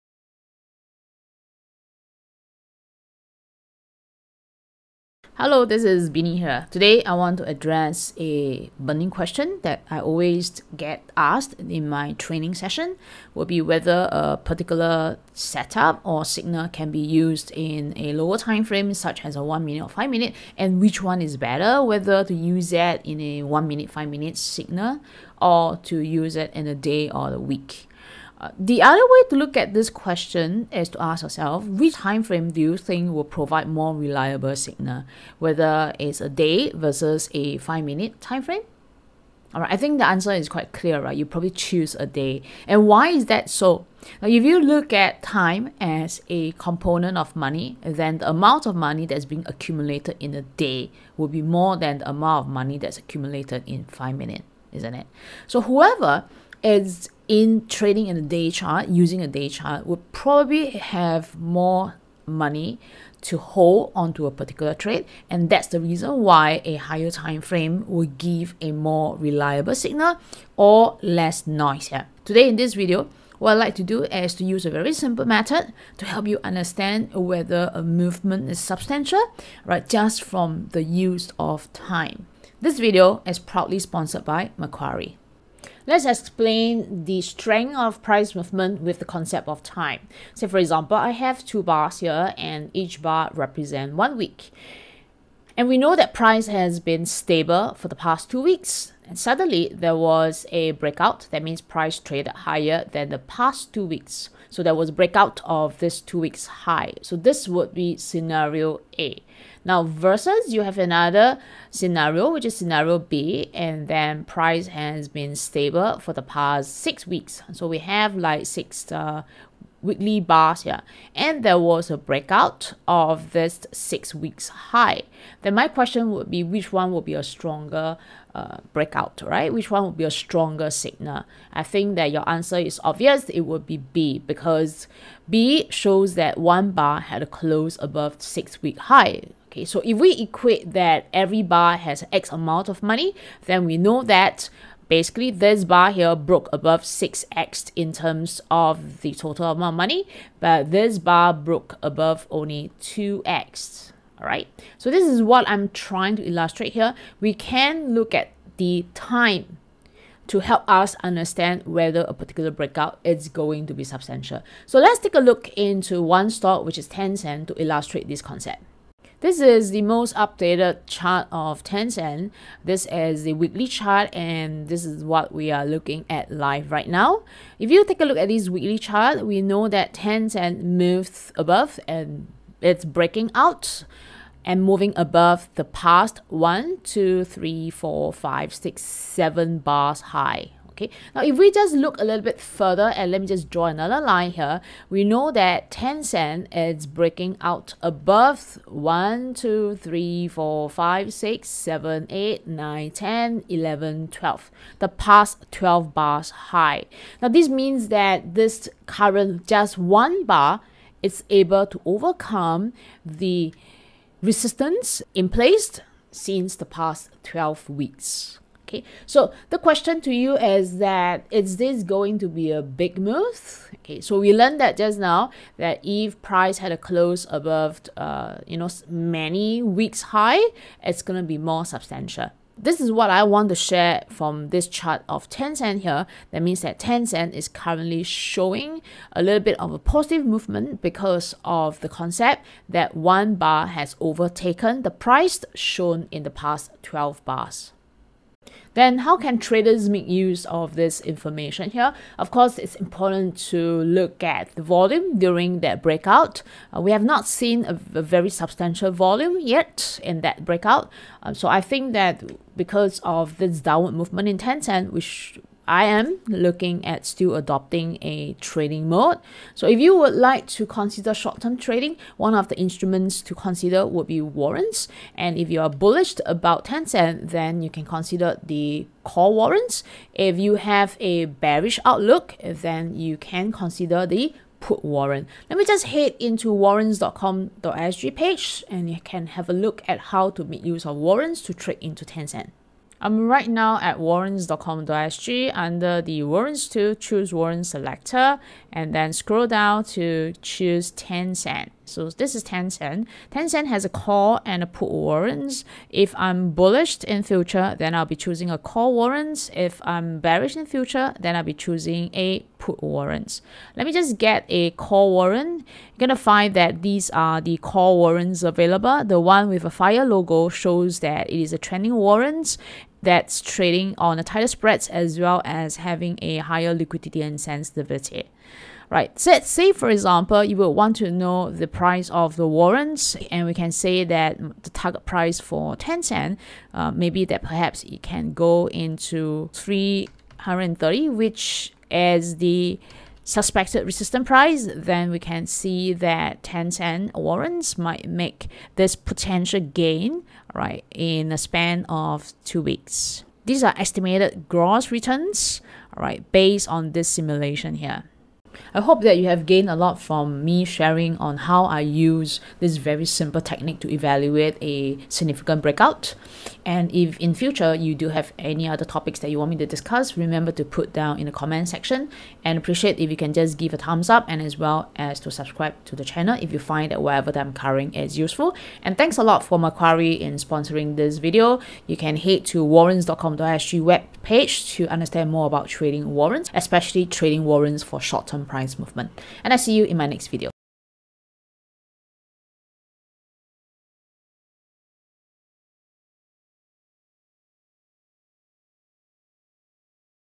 Trading Simplified: One Trick for Powerful Breakouts
⬆️ Master the art of breakouts with this ONE SIMPLE trick. See it in action with REAL data from Tencent Stock!

00:00 Introduction
00:31 Which timeframe would provide more reliable signal?
01:26 Concept of time explanation
02:49 Tencent chart
04:09 How to use this information
04:53 warrants.com.sg
05:58 Ending remarks